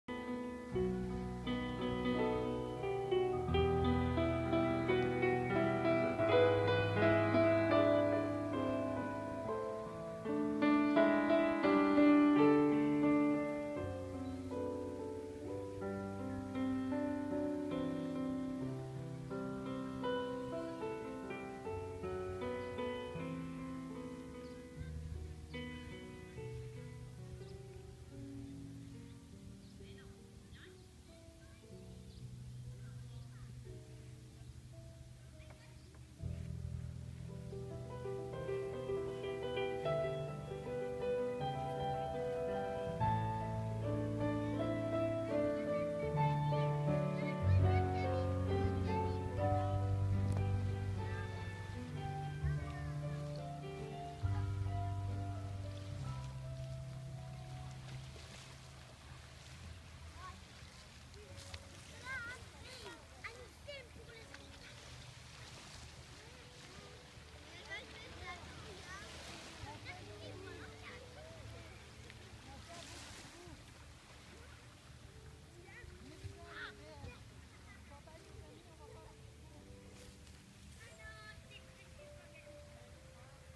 Le 28 juillet 2015 au Havre Musical de l'Islet:Isle aux Coudres
Vidéo amateur d'un après midi au Havre Musical de l'Islet à l'Isle-aux-Coudres dans Charlevoix. Un paradis sur une presqu'ïle en plein milieu du fleuve St-Laurent. 17 chambres en bord de mer.